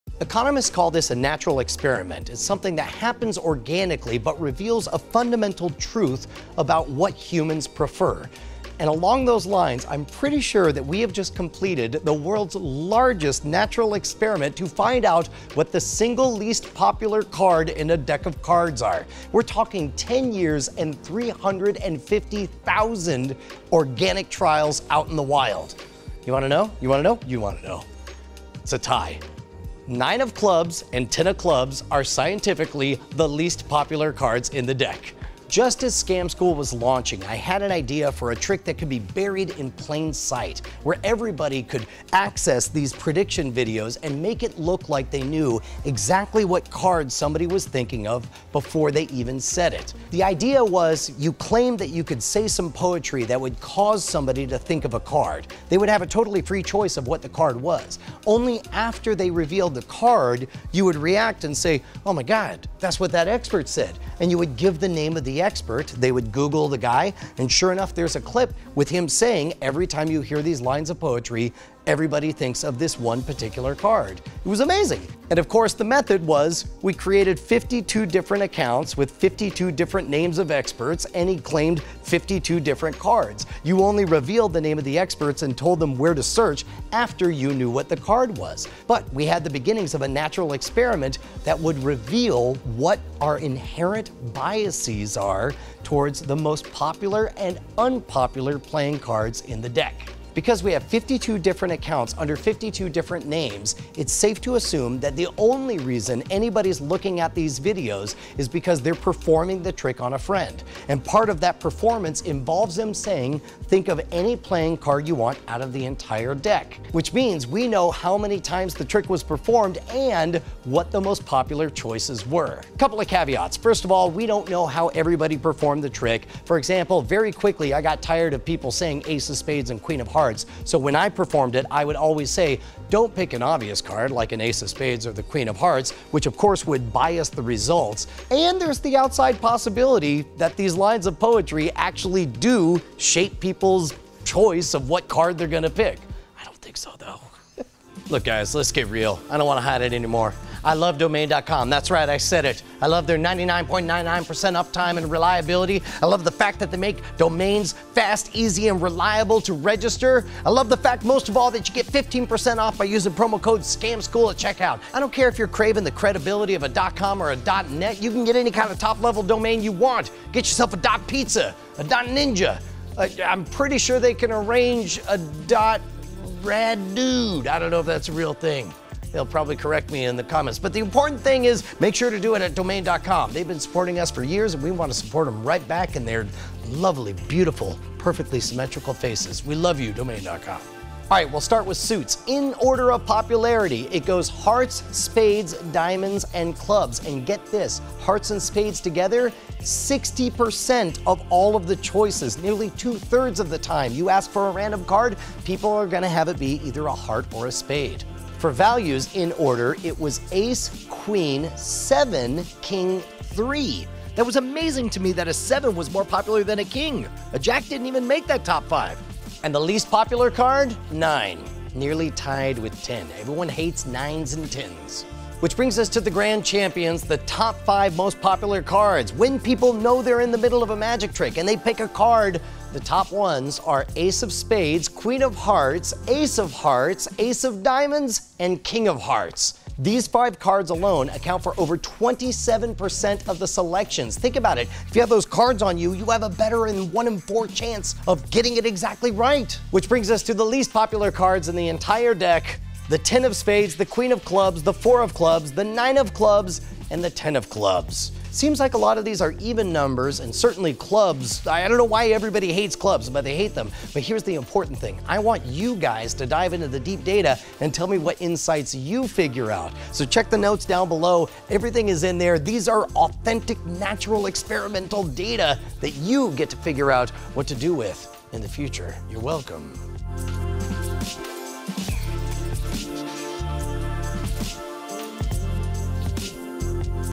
Proven By Science: The Least and Most Popular Cards | Mind Control Scam Data
One of the earliest Scam School tricks has spent the past ten years colelcting valuable data about what are the most and least likely playing cards picked given a free choice. Brian talks through the highs and lows of cards, values, and suits.

Find any good insights in the data? Just got a question or something to send in?

Follow Brian online!

This video made by:
Brian Brushwood
Bryce Castillo
Roberto Villegas
John Rael
Emily Wright